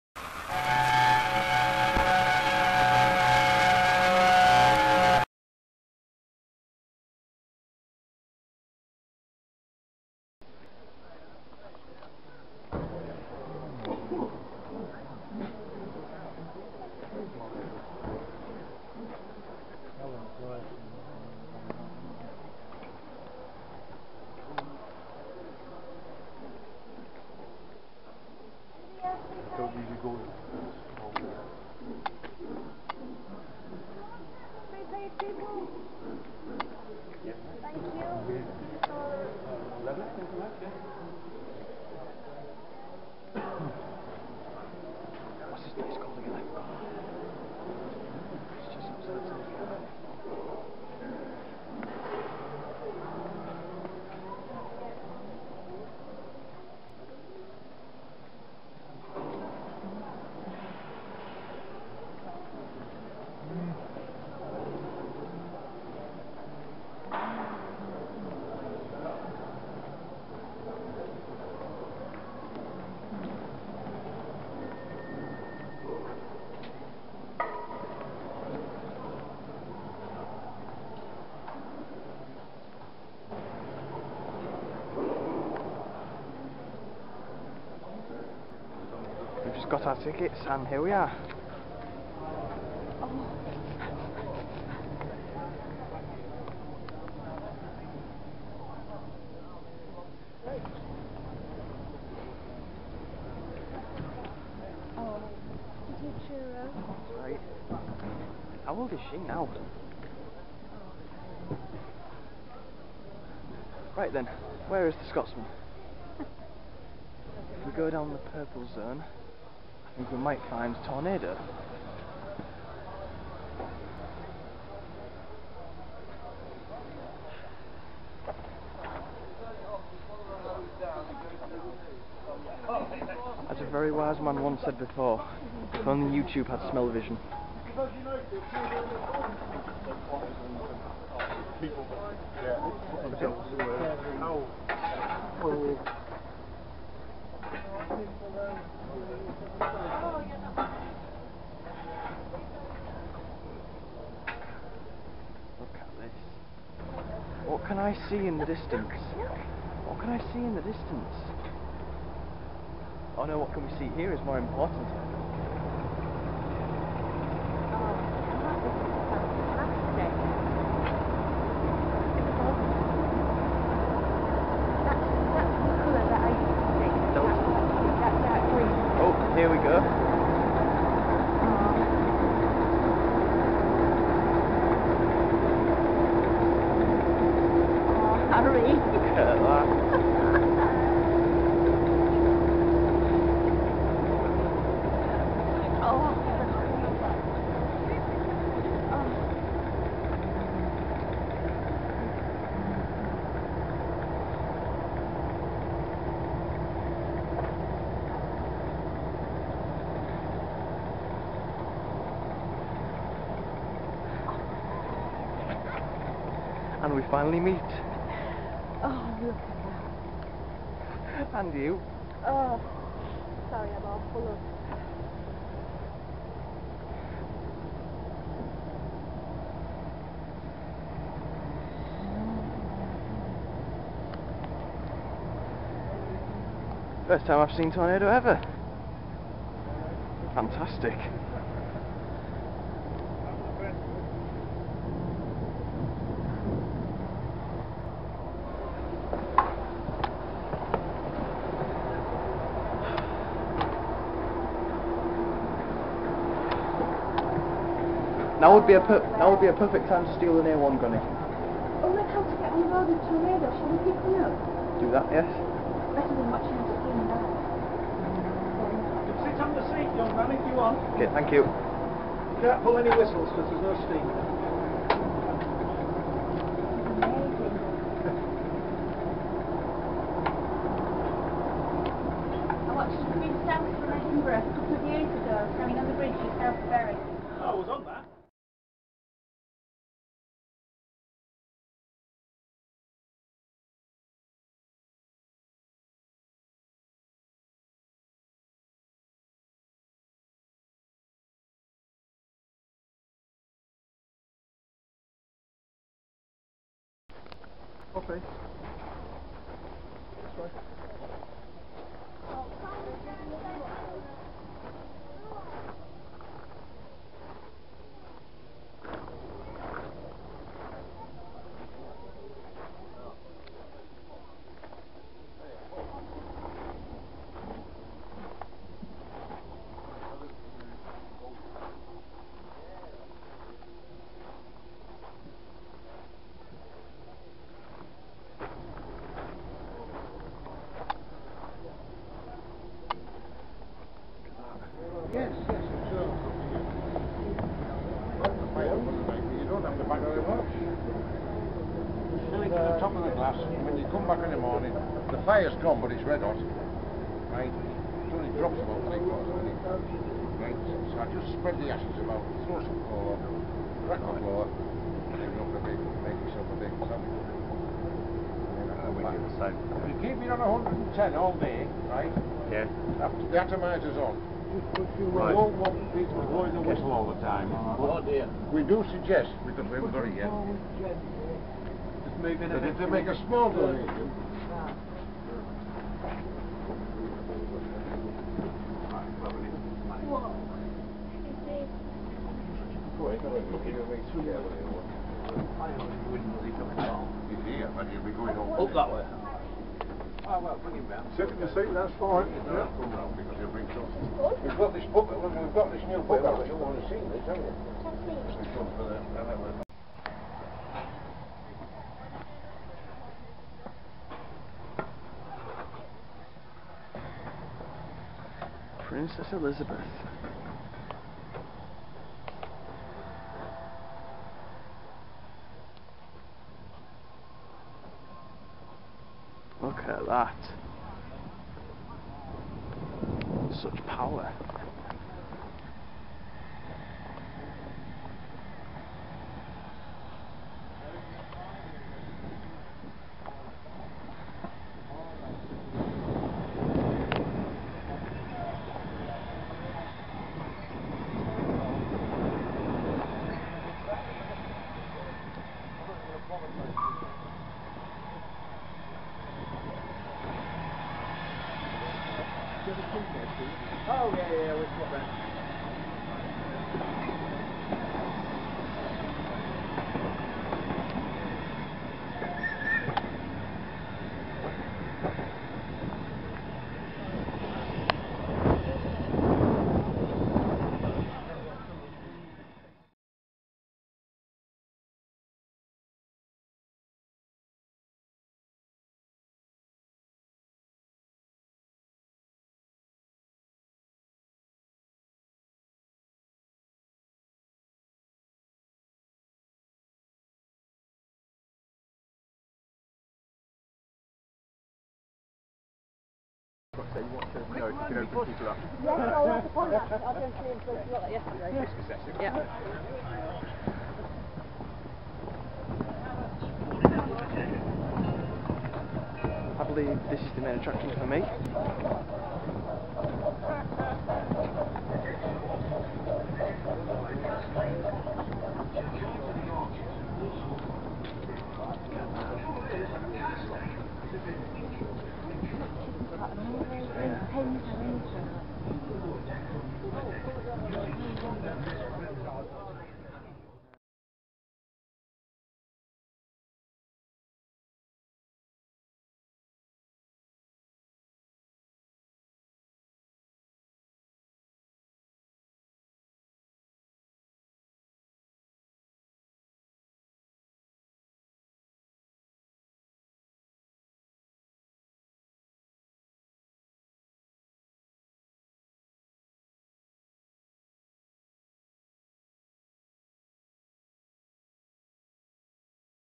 Railfest 2012 Part 1
The breathtaking Railfest 2012, its only once every 6 years so go NOW! It was a spectacular day having everything in one place, its an enjoyable day for all ages and great for getting awesome pictures as you will see.